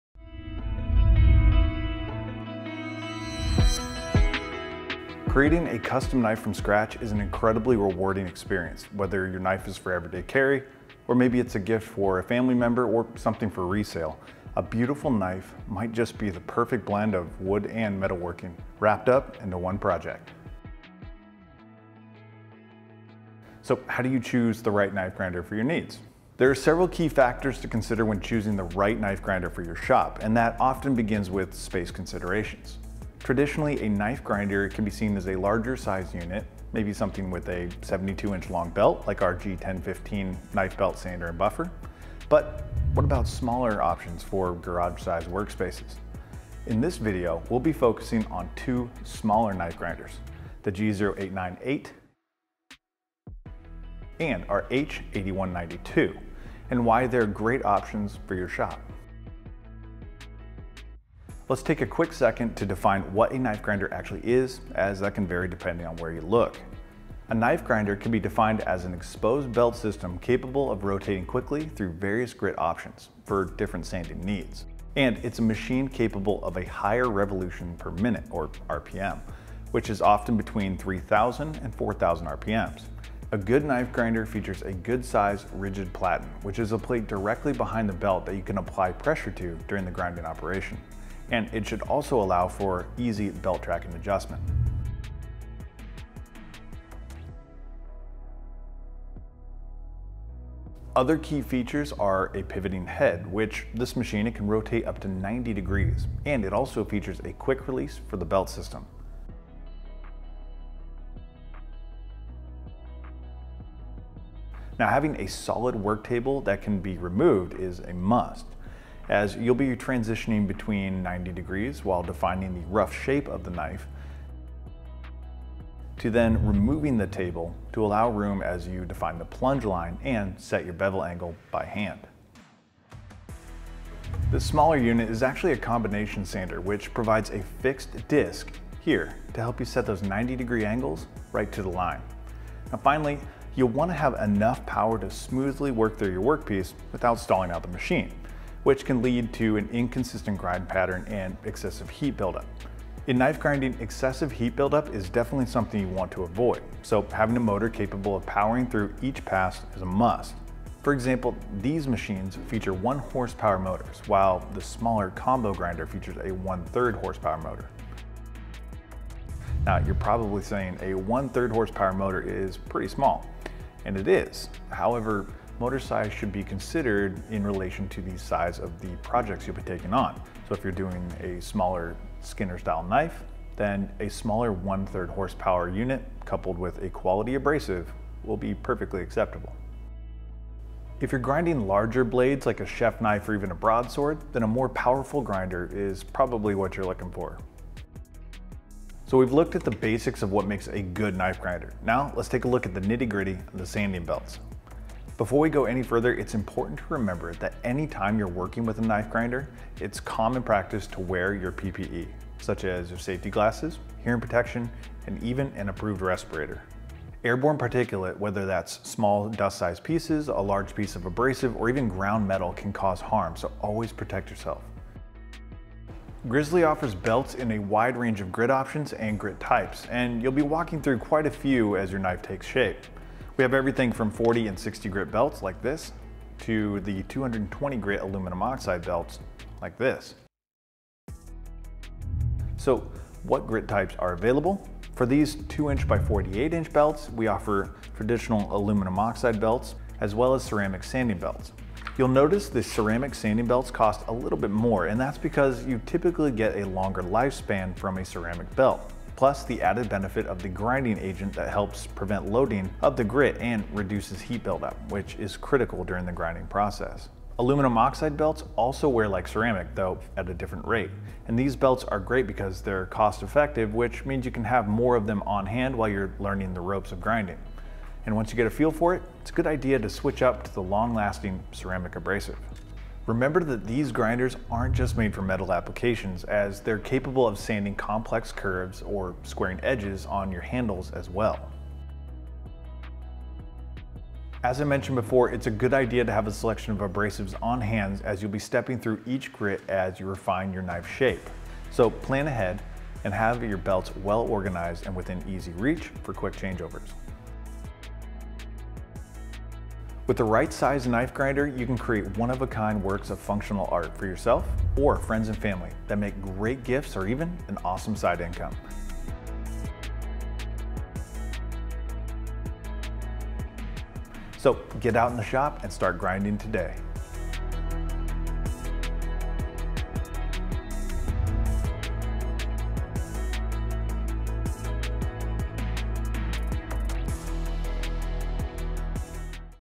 Knife Grinder Overview | Grizzly Industrial
Looking for the perfect blend of woodworking and metalworking? Try knife making!

Creating a custom knife from a solid piece of steel to a polished gift or item for resale is an incredibly fulfilling task, and it's easier than you might think. Grizzly Industrial provides knife grinders and accessories in all the proven sizes to fit your needs, space and budget.

In this video we focus on the grinder styles and uses, then dig into the grinder itself and look at key components that are must-have's. Finally, we take a closer look at grinding belt selection for your projects.

All grinding belt options shown, and much, much more, are available on grizzly.com. Simply search the size belt (length) and grit preference and you're good to grind!

Grizzly Industrial. Quality machines at great prices. Since 1983.